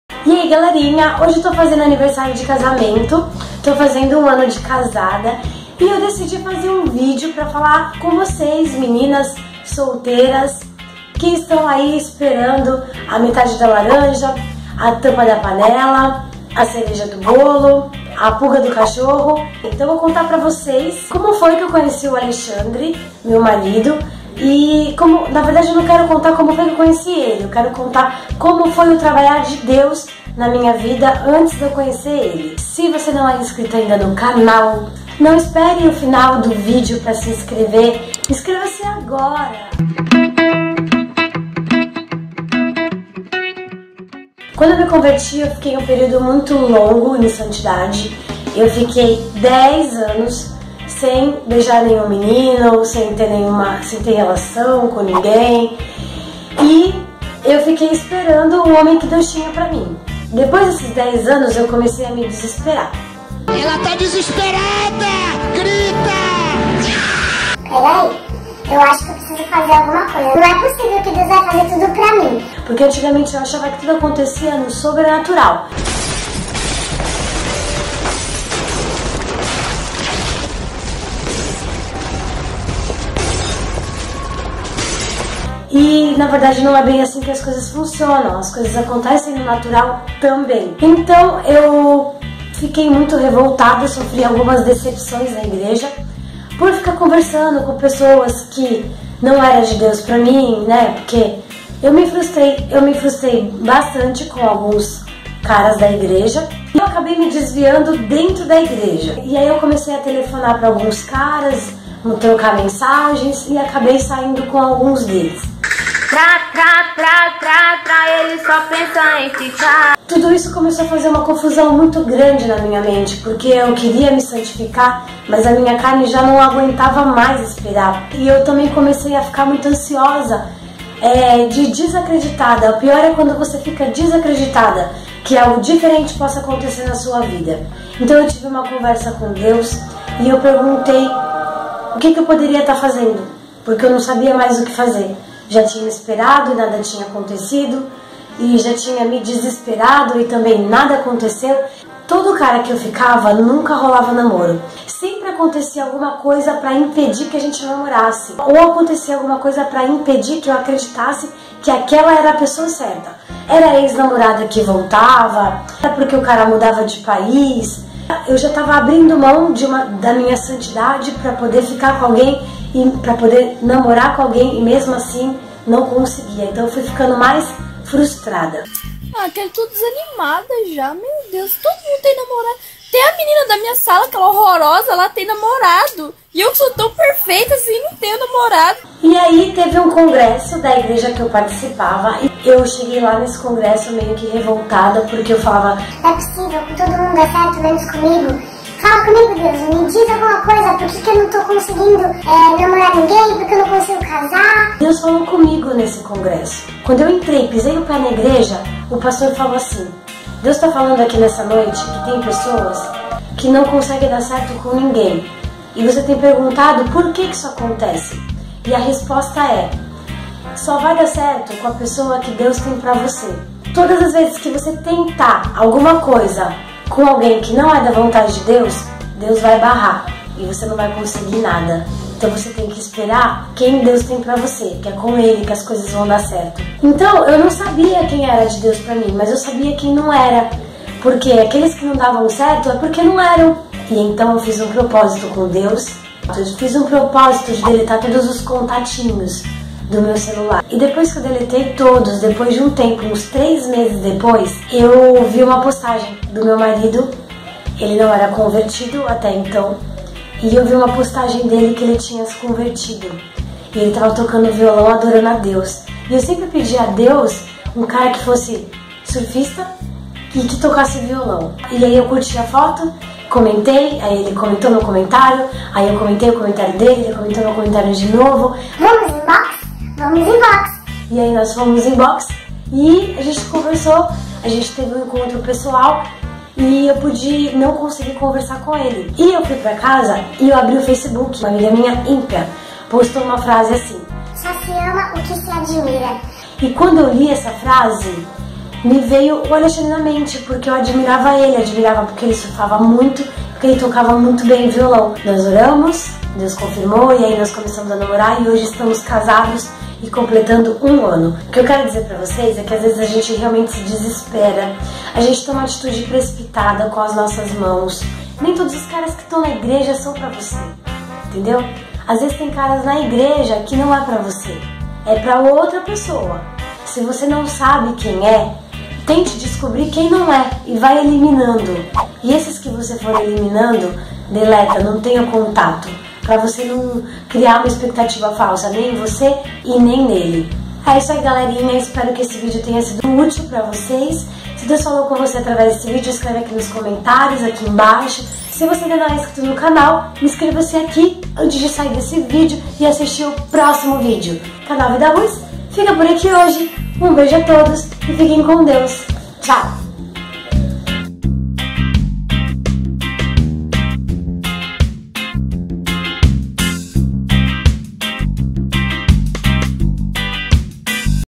Como conquistar um garoto cristão ?
E aeee galerinhaaa !!!
Meninas que estão cansadas de esperar no Senhor e o varão nunca vem !!!
Calma ... nesse vídeo eu conto pra vcs um pouquinho do que passei até conhecer meu boy unção !
Assistam e se INSCREVAM ;)

garoto cristão
boy unção